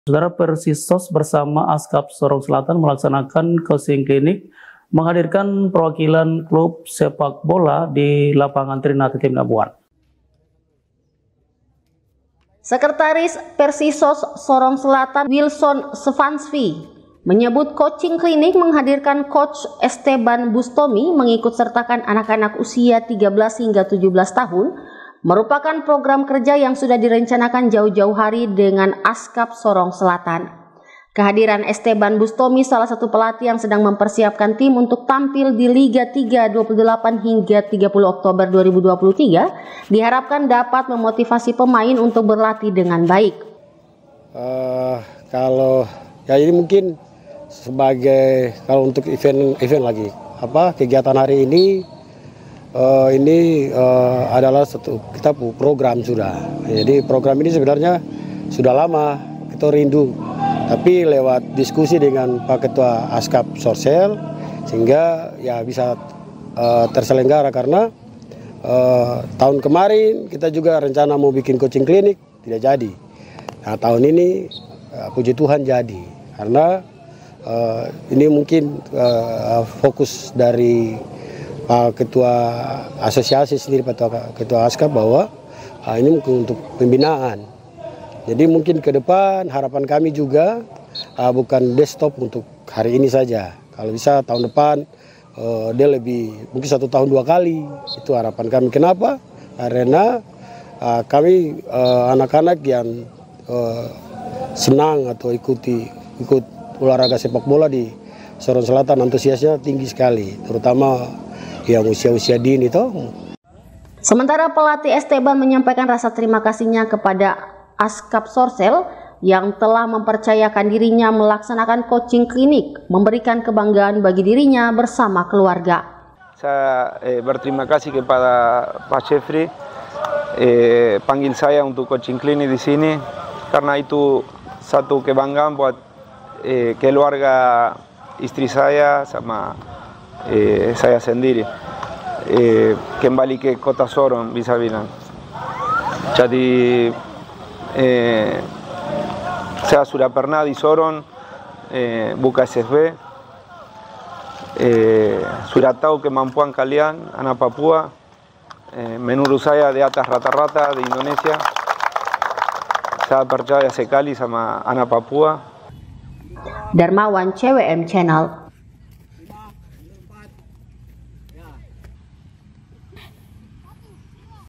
PERSISOS & ASCUP SORSEL GELAR COACHING CLINIC
PERSISOD BERSAMA ASCUP SORONG SELATAN MELAKSANAKAN COACHING CLINIC MENGHADIRKAN PERWAKILAN KLUB SEPAK BOLA DILAPANMGAN TRINATI TEMINABUAN